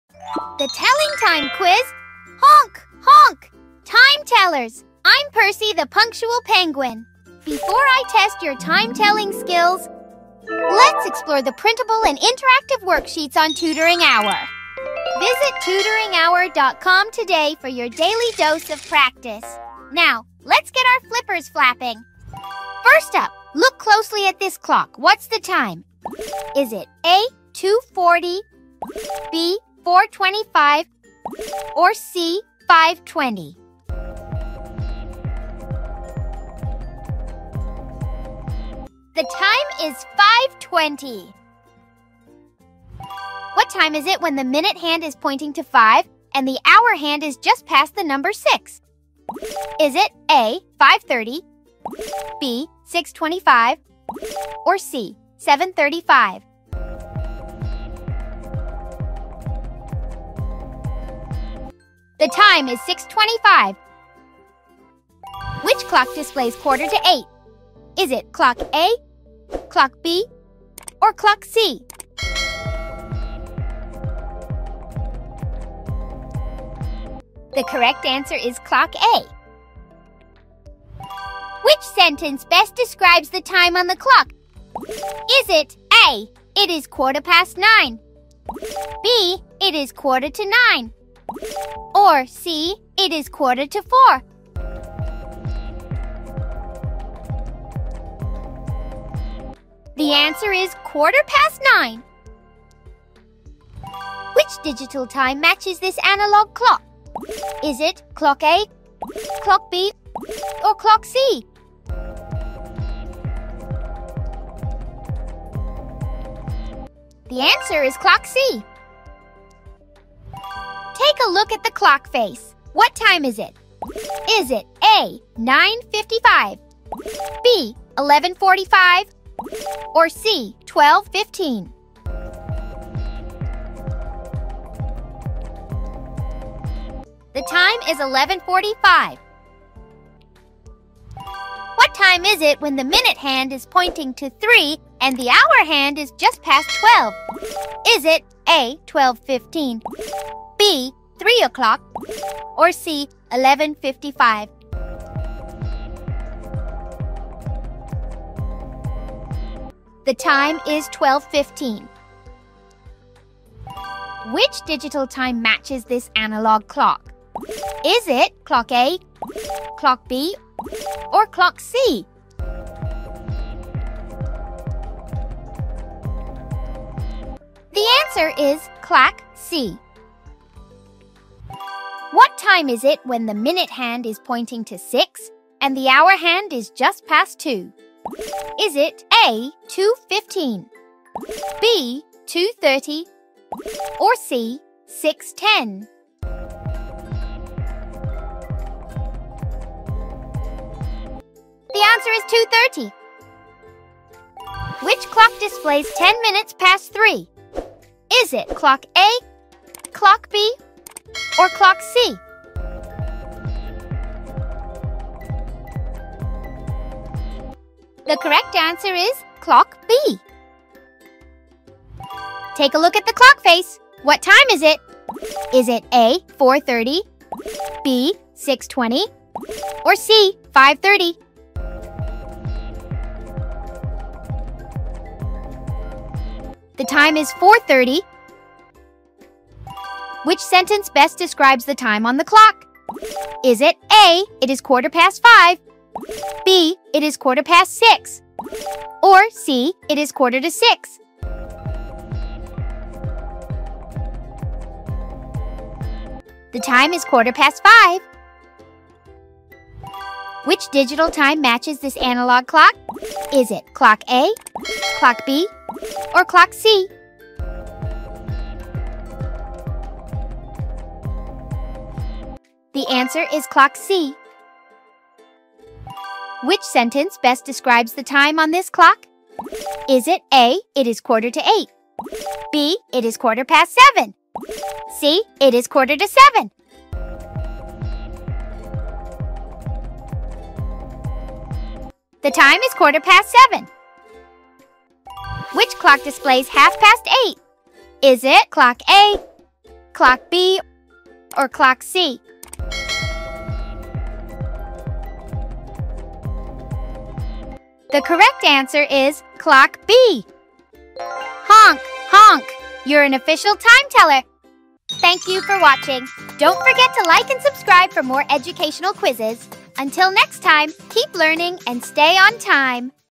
Telling Time Quiz for Kids | Fun Clock Reading Adventure with Percy the Penguin – Honk! Honk!
In this engaging telling time video, Percy the Punctual Penguin will guide you through varied question formats designed to strengthen your understanding of analog and digital clocks. The questions include multiple-choice formats where students must identify the correct time based on the position of the hour and minute hands, select the matching digital time for a given analog clock, and determine which clock corresponds to a specific time expression like "quarter to eight" or "half past eight." Other questions challenge viewers to interpret the time through descriptive sentences such as "It is quarter past nine," asking them to choose the sentence that best matches the time shown. These varied formats help reinforce key time-telling concepts in fun and interactive ways, combining visual recognition, interpretation, and vocabulary associated with time.